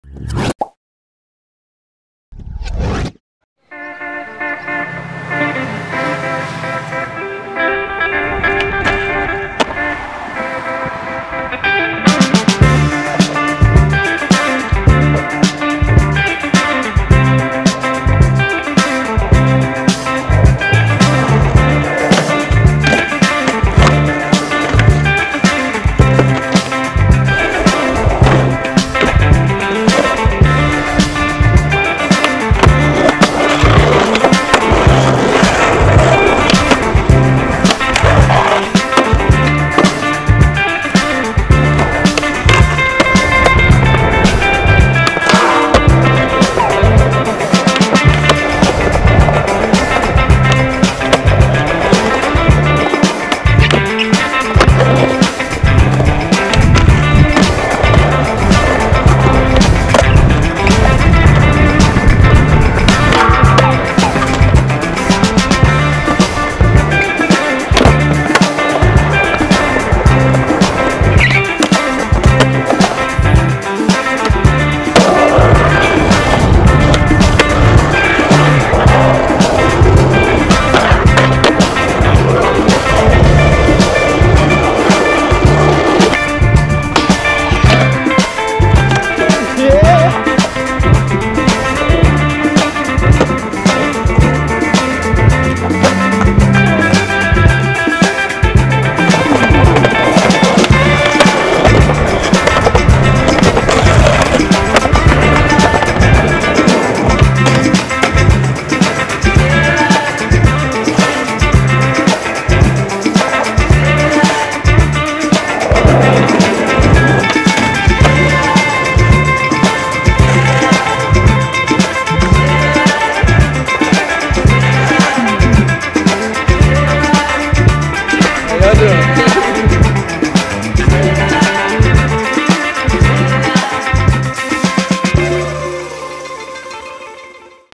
sml. wheels : Tom Knox LA vibes
Some footage of Tom from a few summers ago in LA.

Facebook.com/smlwheels